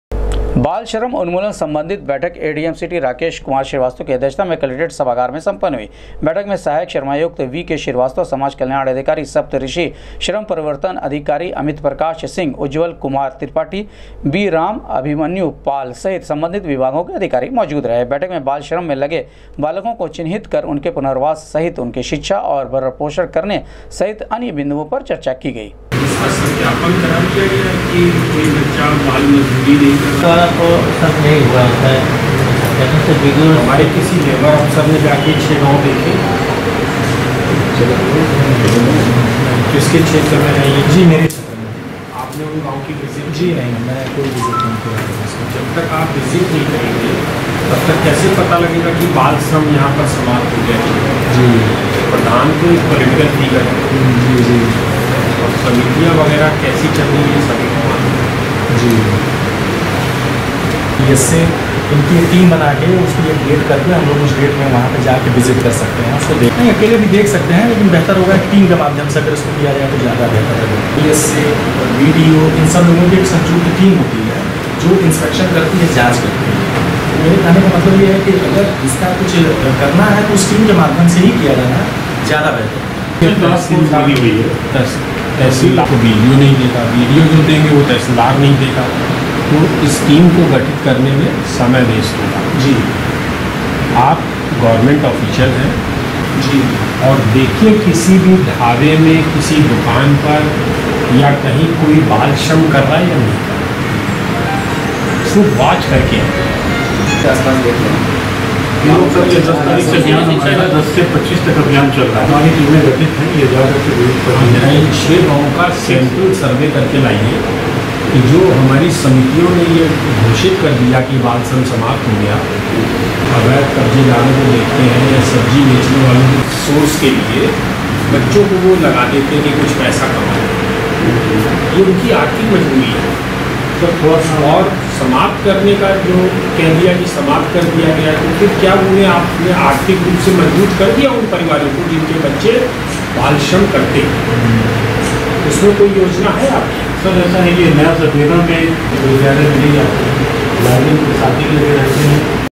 एडीएम सिटी की अध्यक्षता में सम्पन्न हुई बाल श्रम उन्मूलन सम्बन्धित बैठक,बाल श्रमिकों को
एडीएम सिटी की अध्यक्षता में सम्पन्न हुई बाल श्रम उन्मूलन सम्बन्धित बैठक,बाल श्रमिकों को चिन्हित कर उनके पुनर्वास पर की गई चर्चा